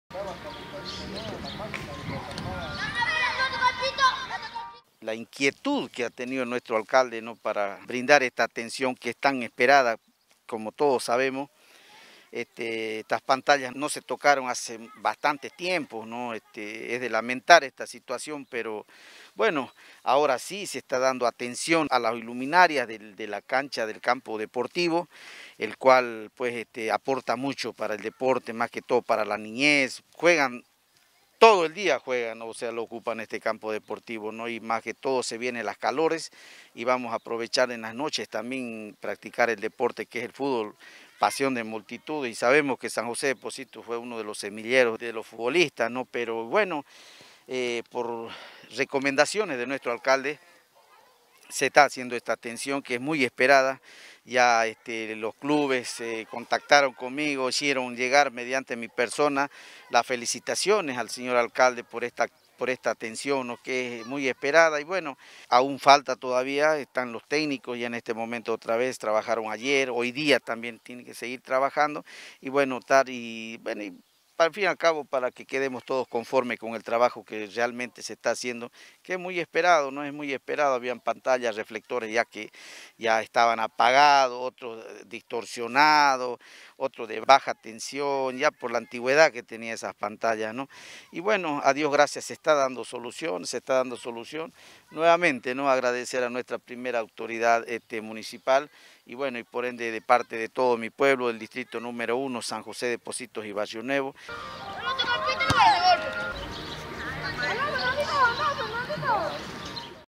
Gobierno Municipal realiza arreglo y mantenimiento de las luminarias del estadio Néstor Paz Galarza
Gobierno Municipal realiza arreglo y mantenimiento de las luminarias del estadio Néstor Paz Galarza de Barrio Nuevo.